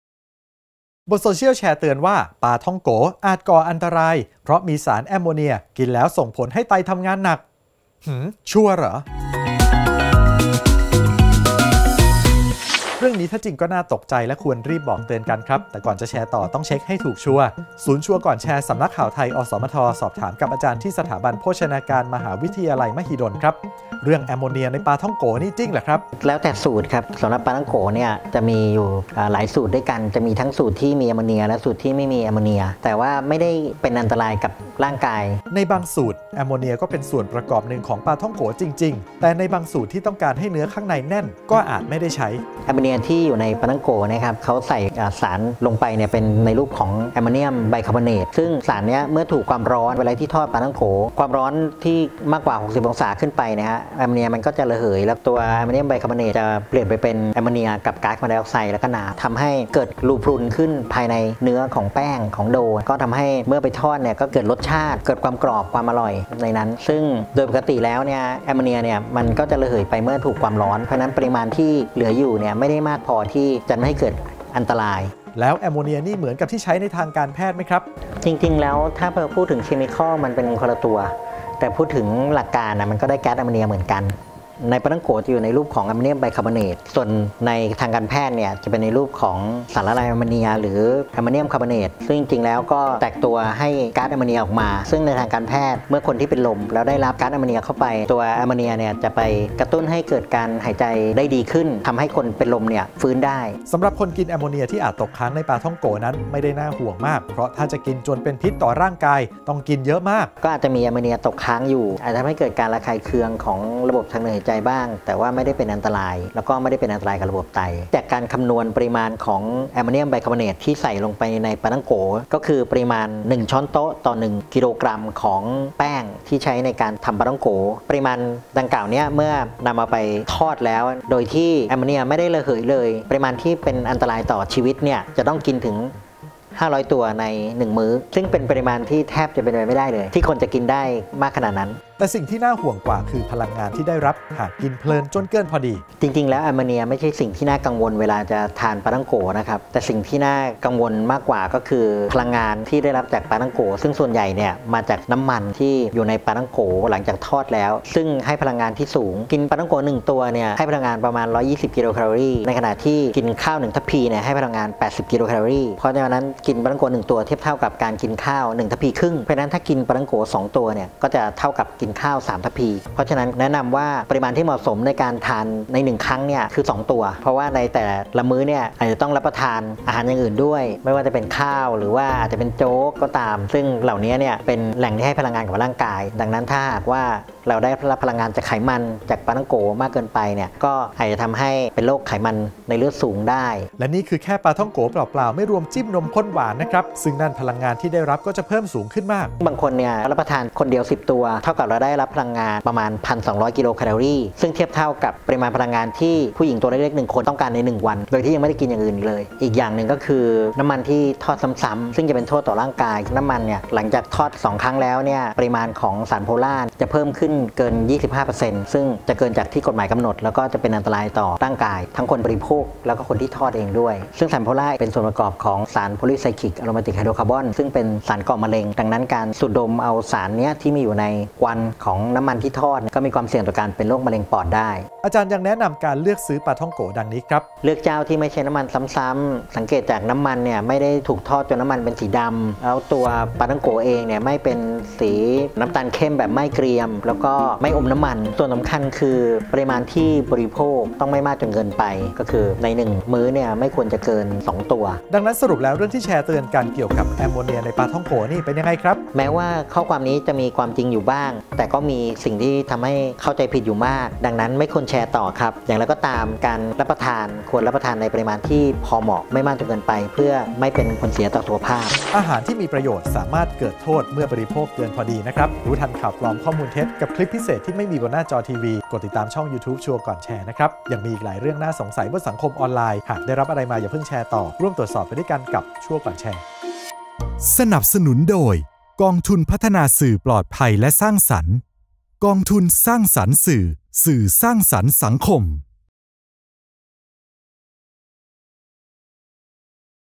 ชัวร์ก่อนแชร์ : ปาท่องโก๋อันตราย มีแอมโมเนีย กระทบต่อไต จริงหรือ ?
บนสังคมออนไลน์แชร์เตือนว่า ให้ระวังการรับประทานปาท่องโก๋ เนื่องจากมีแอมโมเนีย กินแล้วจะส่งผลให้ไตทำงานหนัก เรื่องนี้จริงหรือไม่ ติดตามกับคุณพีรพล อนุตรโสตถิ์ จากศูนย์ชัวร์ก่อนแชร์

🎯 ตรวจสอบกับ รศ.ดร.เอกราช เกตวัลห์
อาจารย์ สถาบันโภชนาการ มหาวิทยาลัยมหิดล

📌 สรุป : ❌ ไม่ควรแชร์ต่อ ❌
แม้ว่าข้อความนี้จะมีความจริงอยู่บ้าง
แต่ก็มีสิ่งที่ทำให้เข้าใจผิดอยู่มาก
ดังนั้น ไม่ควรแชร์ต่อ
อย่างไรก็ตาม การรับประทานควรรับประทานในปริมาณที่พอเหมาะ
ไม่มากจนเกินไป เพื่อไม่เป็นผลเสียต่อสุขภาพ
อาหารที่มีประโยชน์สามารถเกิดโทษเมื่อบริโภคเกินพอดี

ข่าวค่ำ :: สำนักข่าวไทย อสมท | ช่อง 9 MCOT HD เลข 30